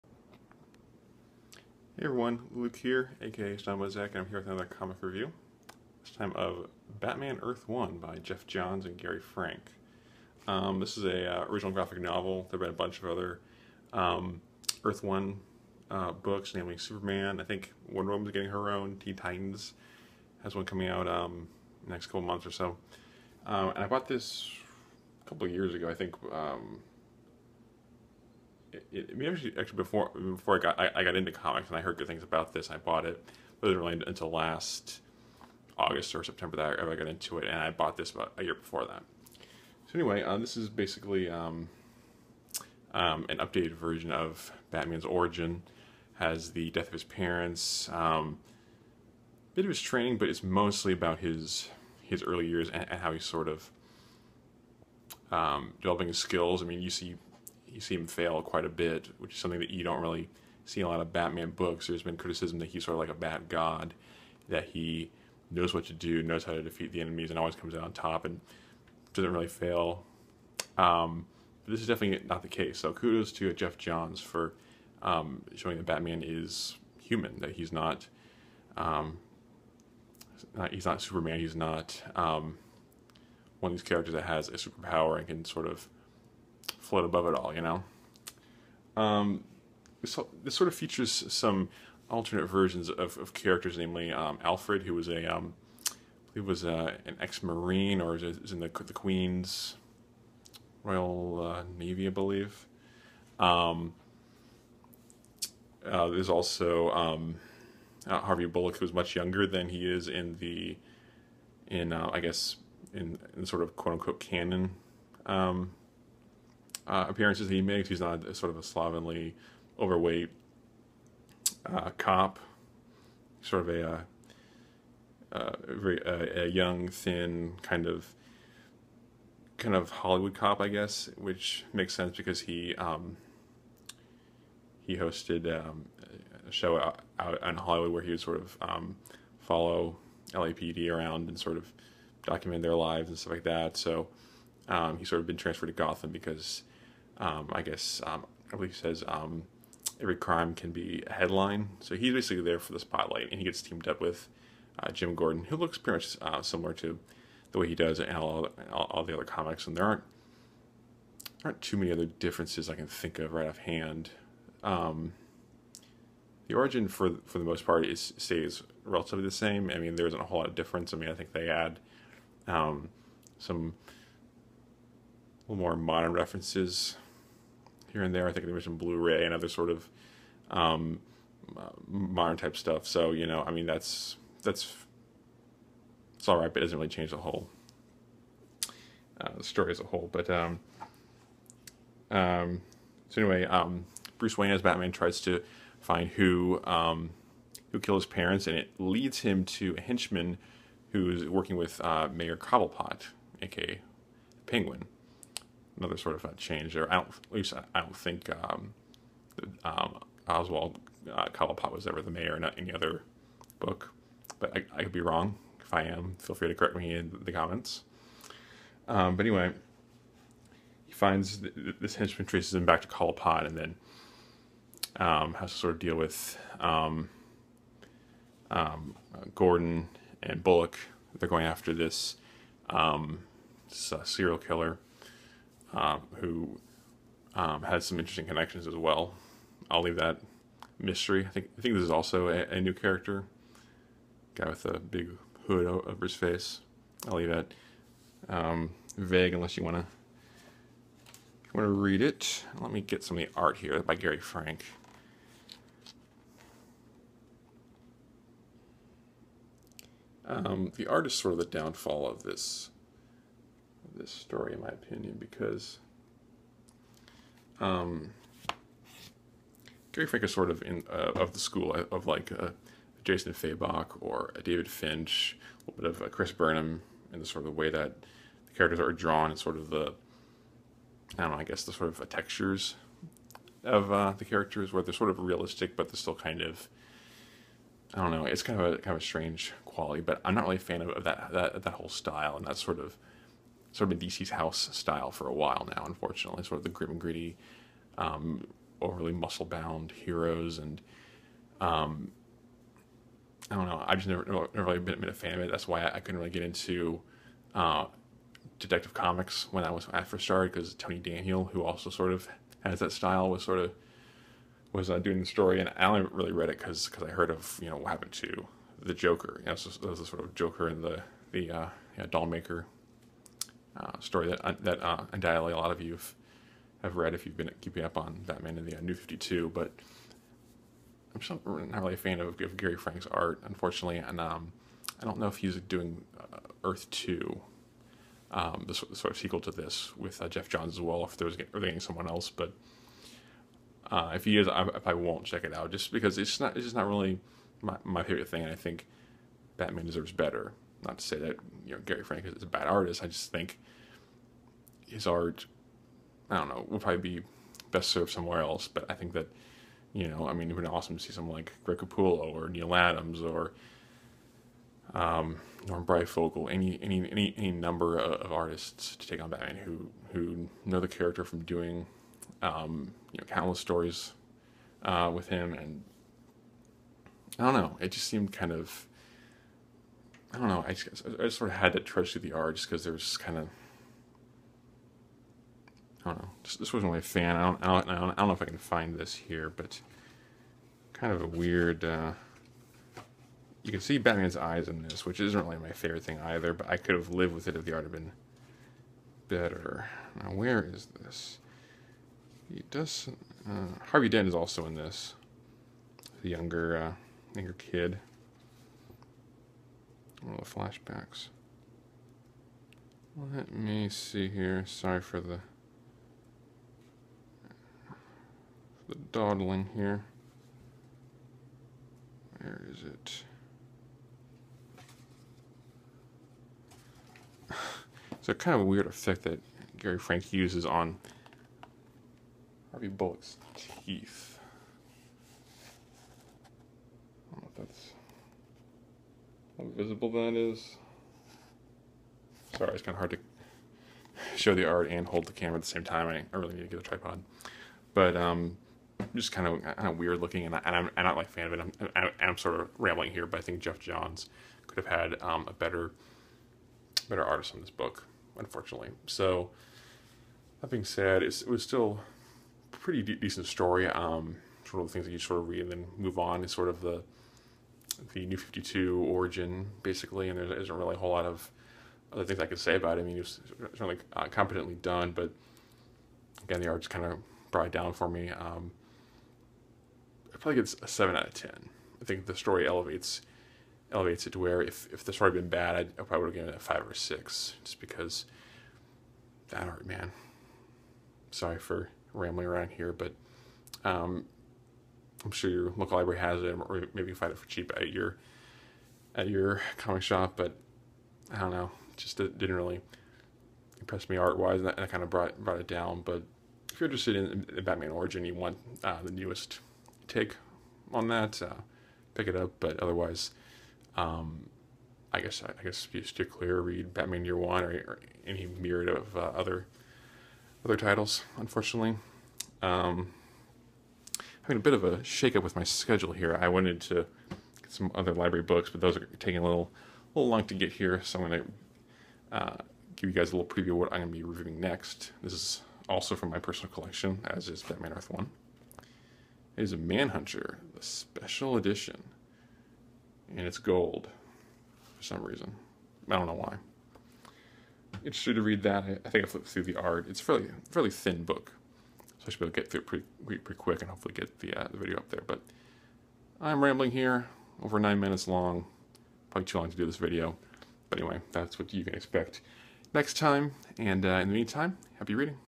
Batman: Earth One Review
Geoff Johns updates Batman's origin story with new takes on old characters, such as a young Harvey Bullock, Mayor Oswald Cobblepot, and ex-Royal Marine Alfred Pennyworth.